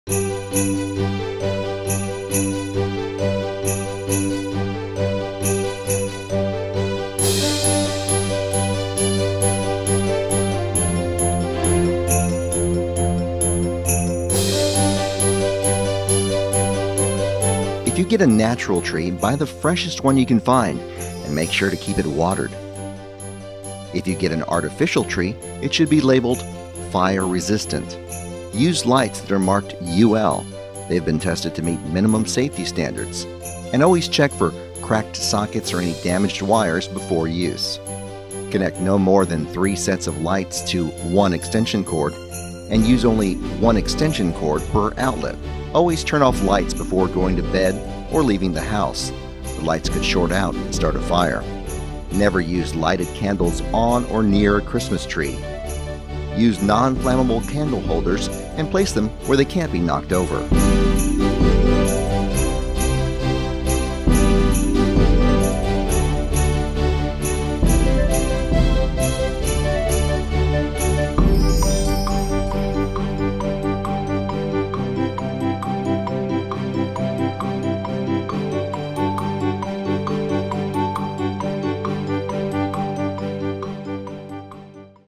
Christmas Tree Safety Tips | Texas State Fire Marshal's Office
A small fire that spreads to a Christmas tree can grow quickly. Here are some tips to keep your family safe as you deck the halls.